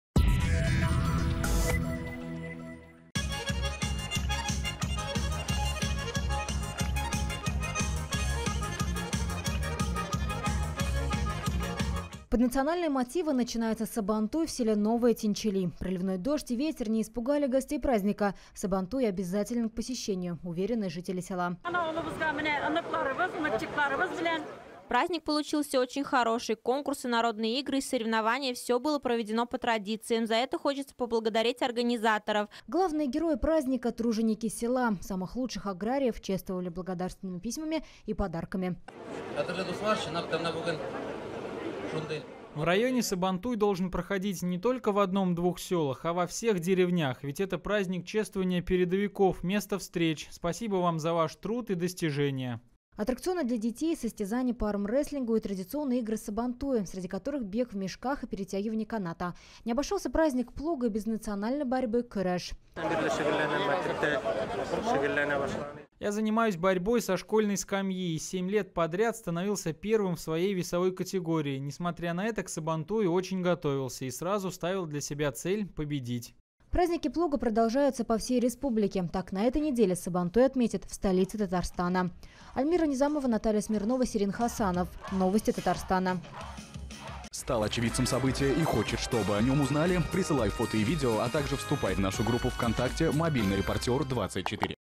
В селе Буинского района отметили Сабантуй
Праздник плуга отметили жители села Новые Тинчали. Гостей праздника ждали народные забавы, конкурсы для детей, татарские песни и традиционные угощения.